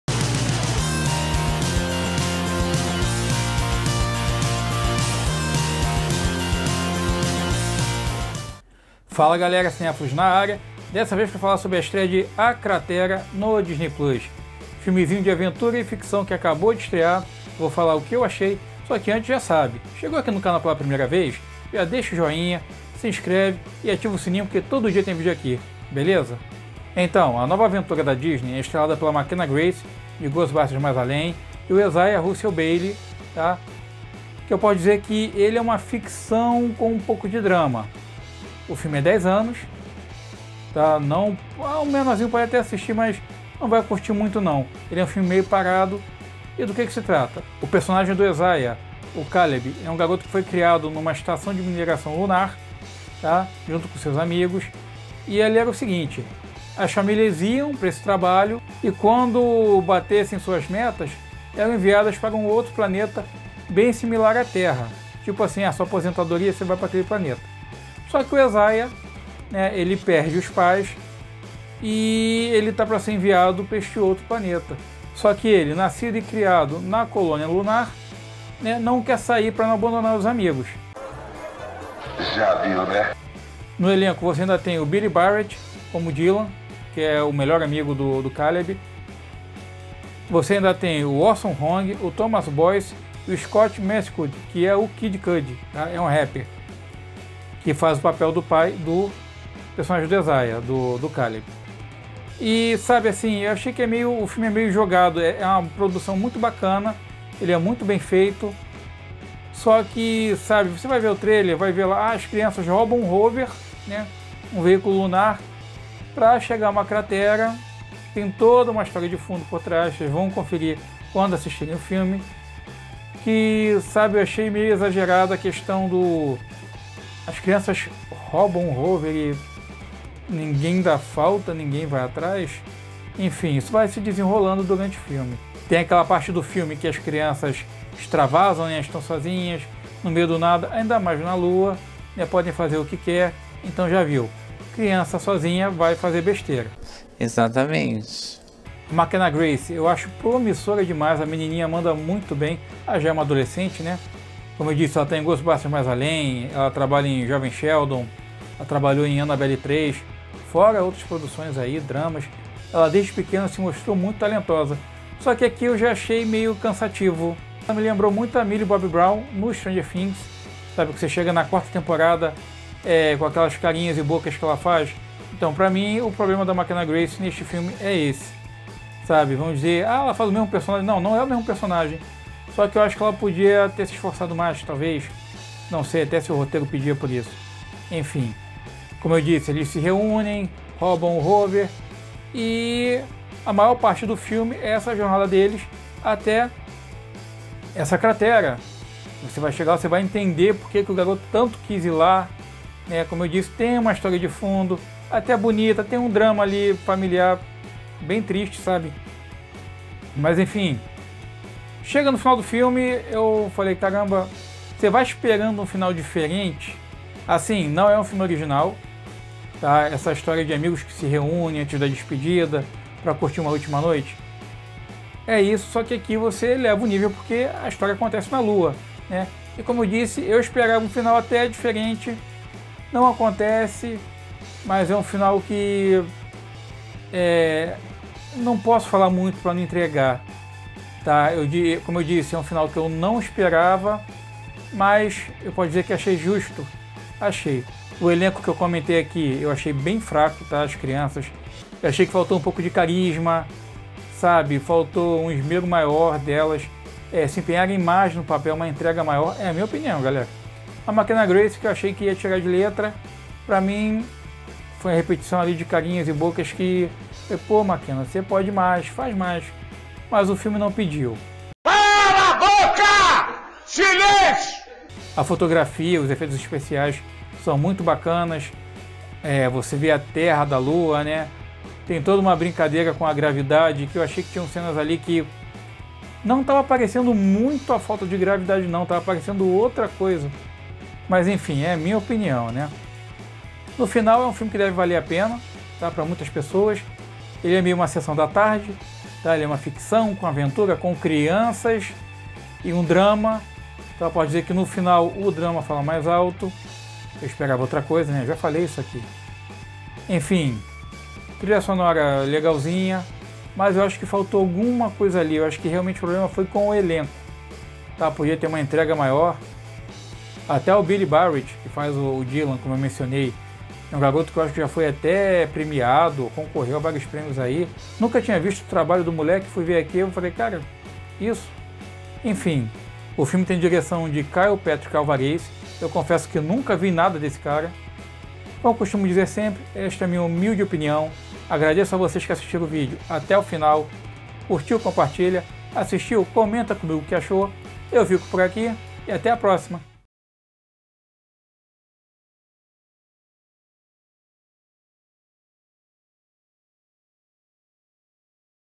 "A Cratera" - Crítica
Estreou no Disney Plus, "A Cratera", ficção e aventura, estrelado por Mckenna Grace e Isaiah Russell-Bailey! Confira!